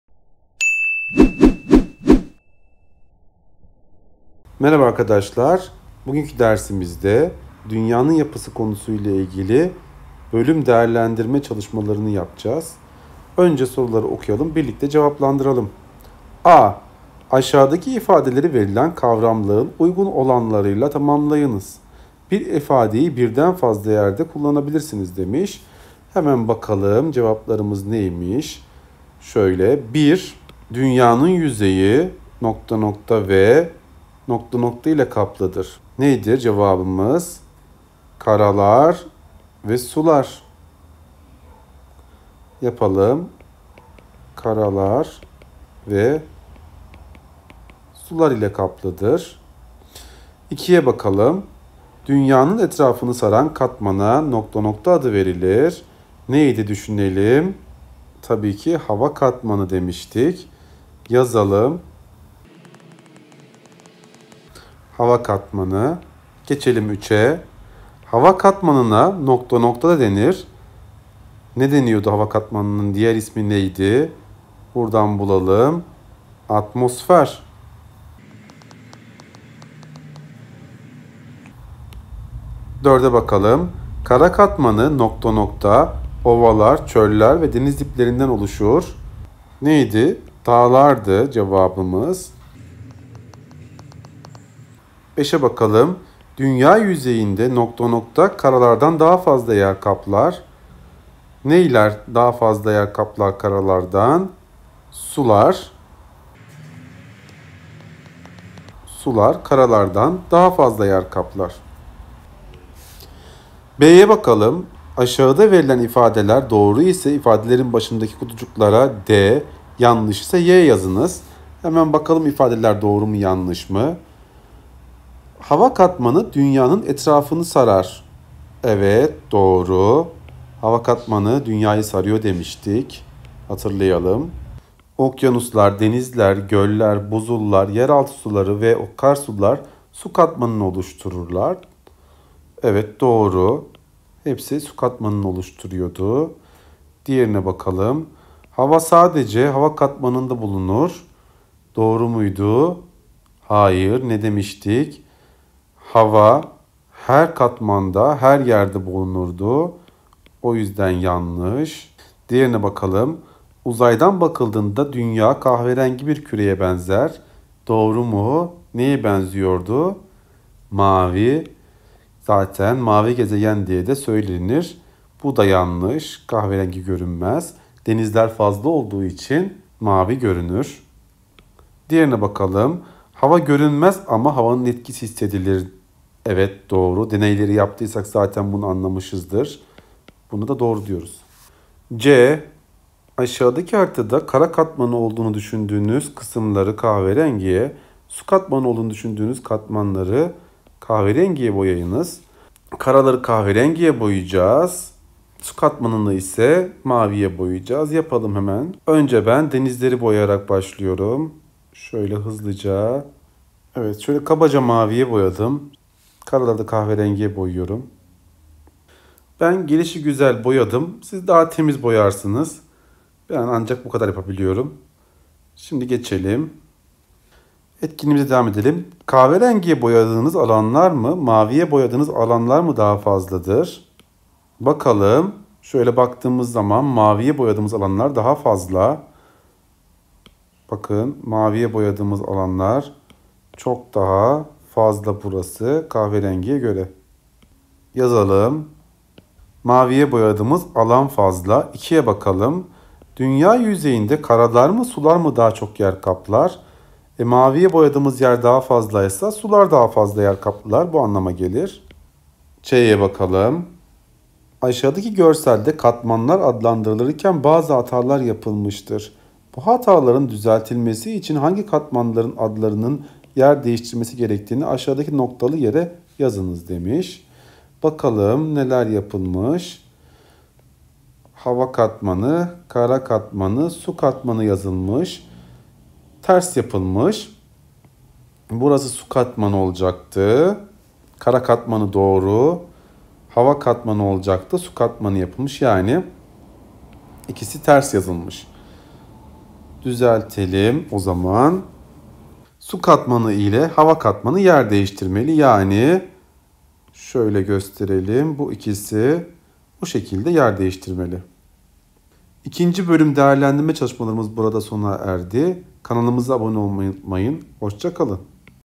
3.Sınıf Fen Bilimleri Ders Kitabı 2.Bölüm Değerlendirme Çalışmaları Cevapları
3.Sınıf Fen Bilimleri Ders Kitabı 2.Bölüm Değerlendirme Çalışmaları Cevaplarını birlikte yapalım.